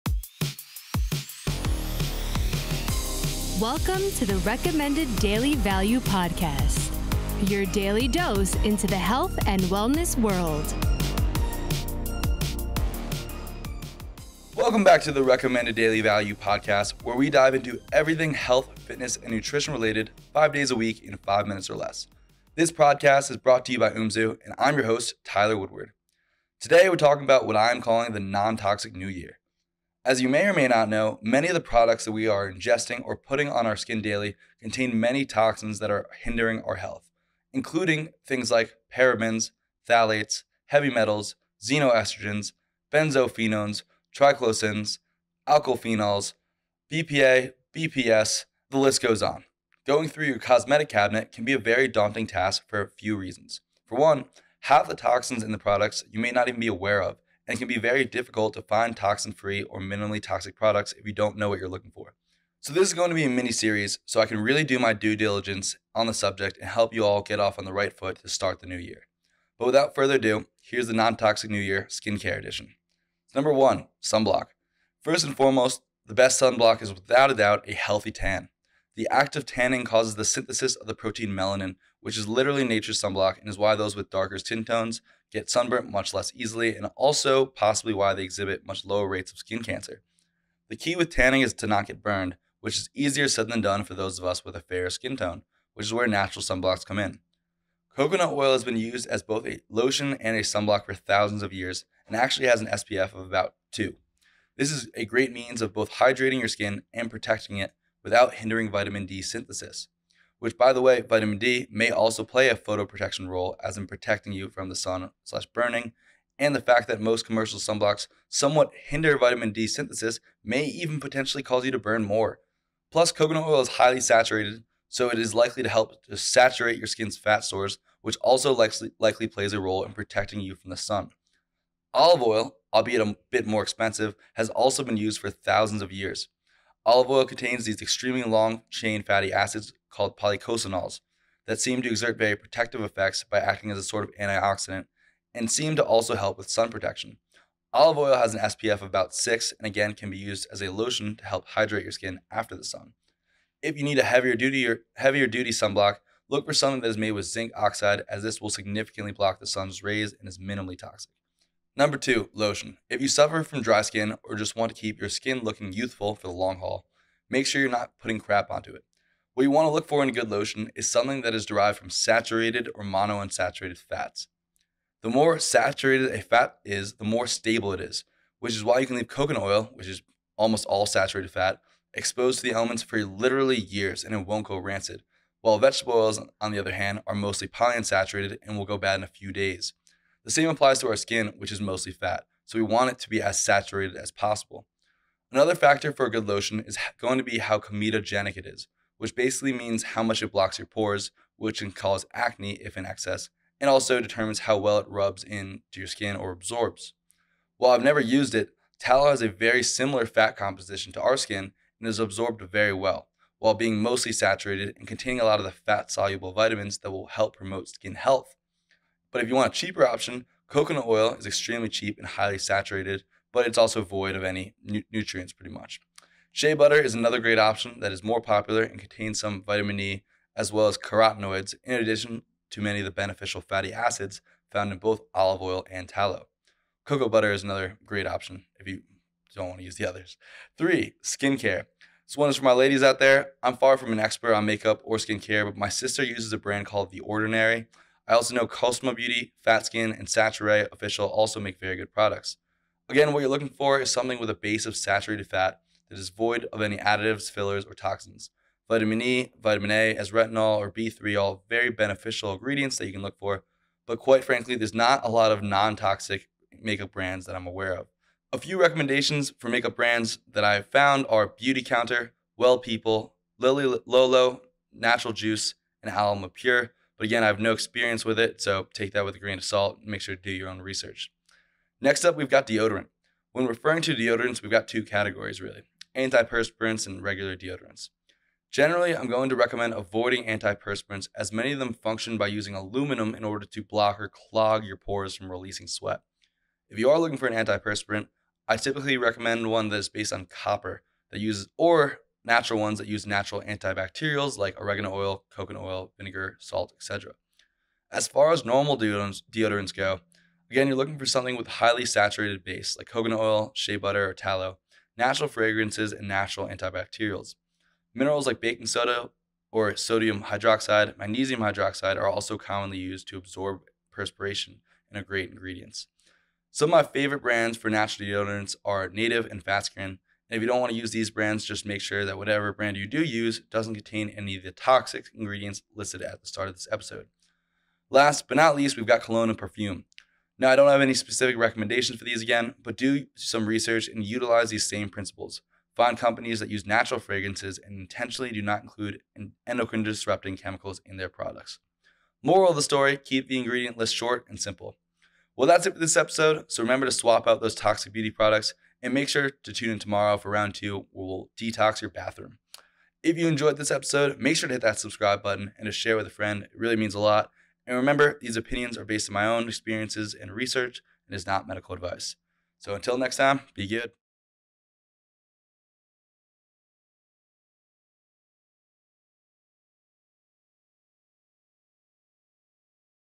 Non-Toxic Beauty Routine RDV #65
In this episode Tyler goes into how to start your New Year off right with non-toxic skincare products including sunblock, lotion, make-up and the principles behind them